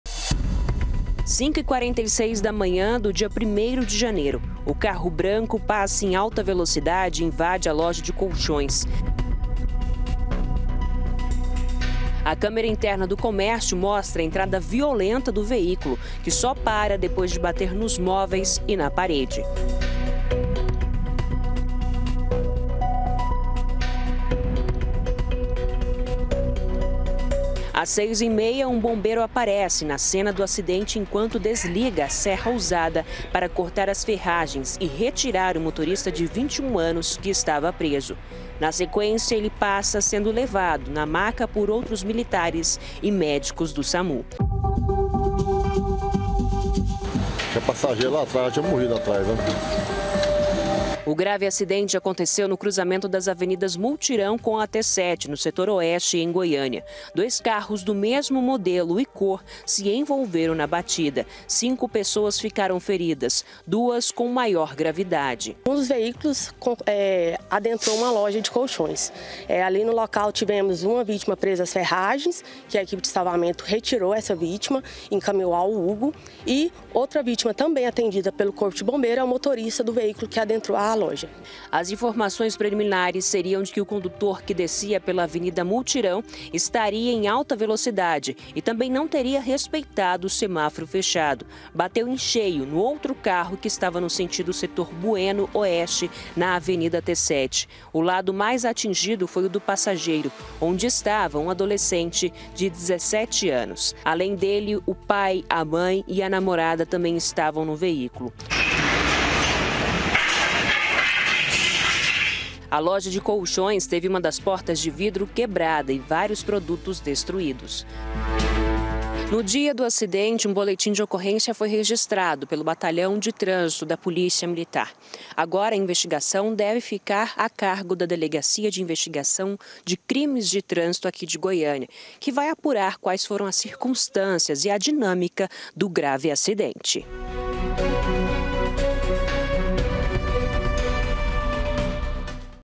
ACIDENTE DE TRÂNSITO: CÂMERAS REGISTRAM MOMENTO EM QUE CARRO INVADE LOJA
CÂMERAS DE SEGURANÇA REGISTRAM O MOMENTO EM QUE UM CARRO INVADIU UMA LOJA DE COLCHÕES DEPOIS DE SE ENVOLVER EM UMA ACIDENTE NA MADRUGADA DO DIA PRIMEIRO. COM O IMPACTO DA BATIDA, O OUTRO VEÍCULO SAIU GIRANDO E SÓ PAROU DEPOIS DE BATER COM A LATERAL NUM POSTE.